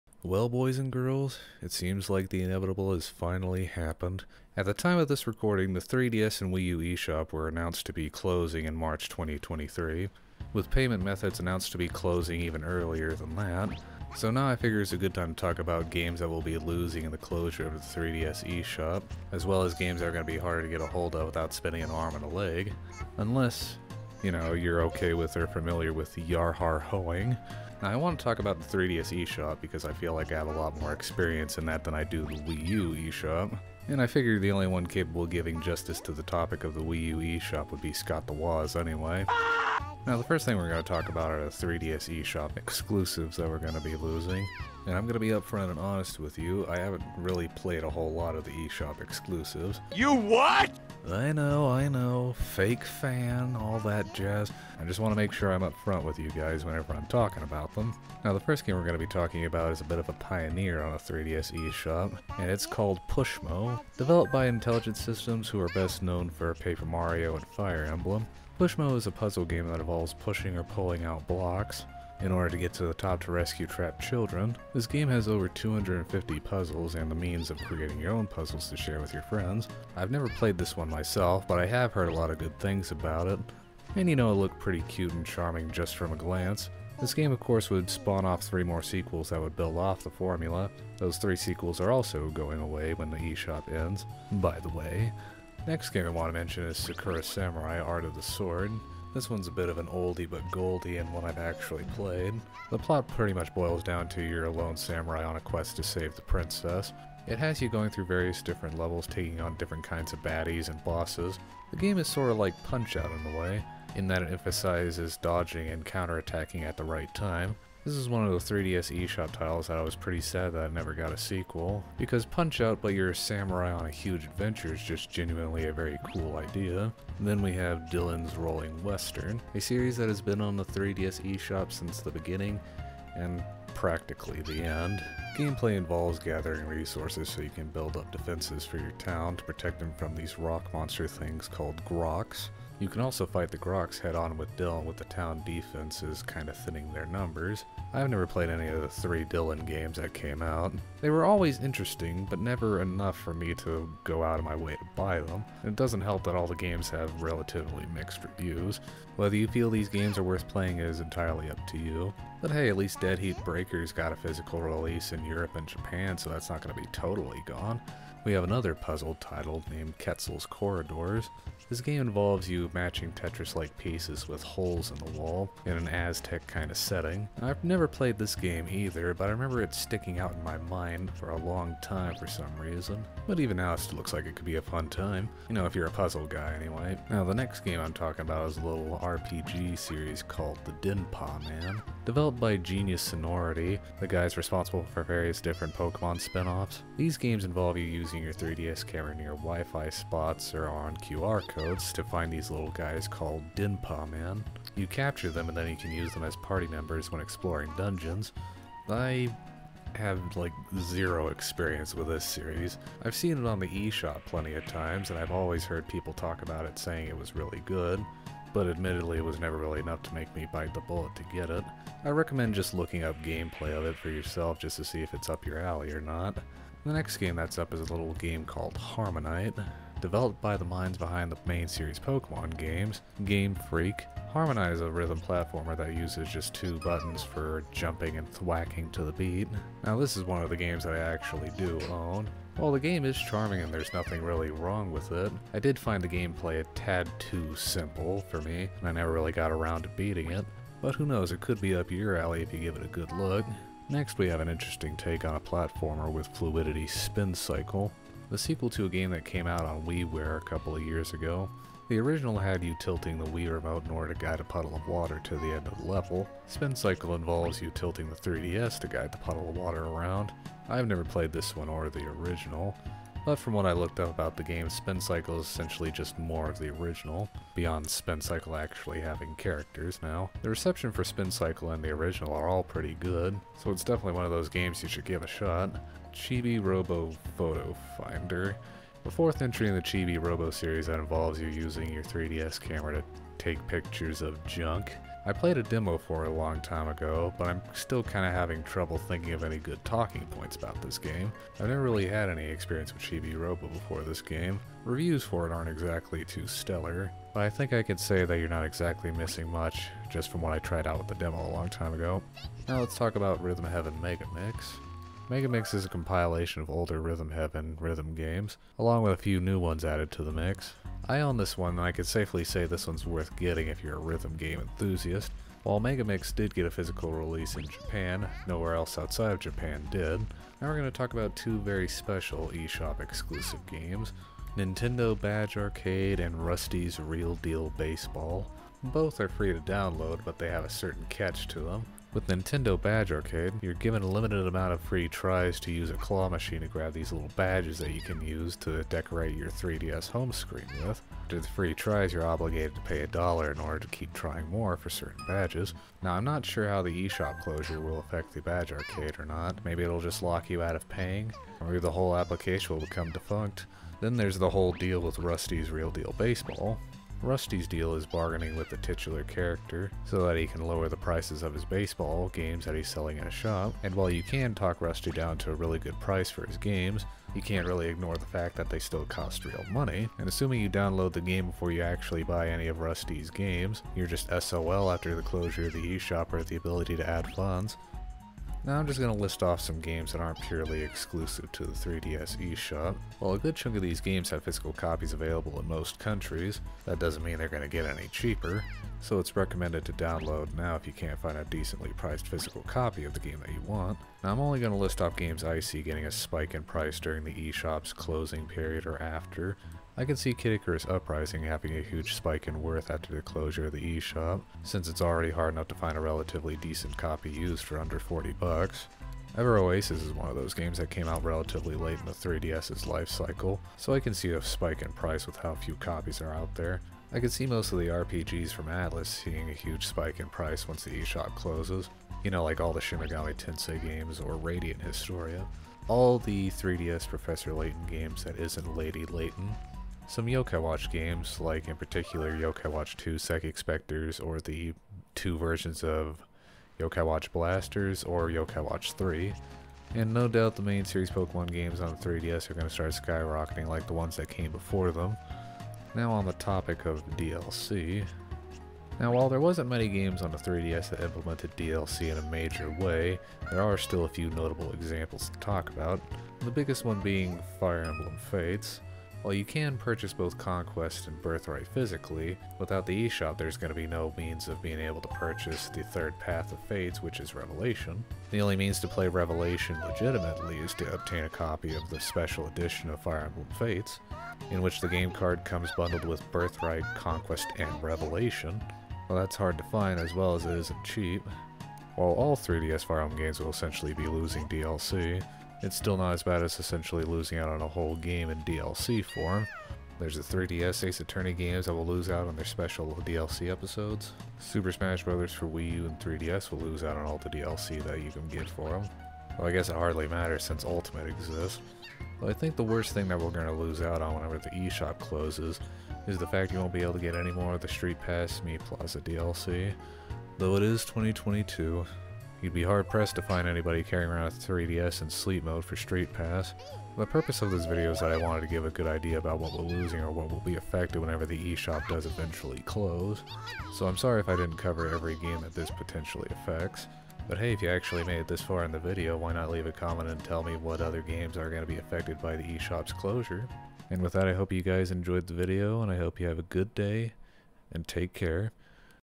The 3DS eShop is Closing - What We'll Lose and What It Means
I figured I'd make a small video talking about the impending closure of the 3DS eShop. I don't go into much detail and cover everything we're gonna lose. I just wanted to give a general idea and try and spread awareness while maybe giving some recommendations.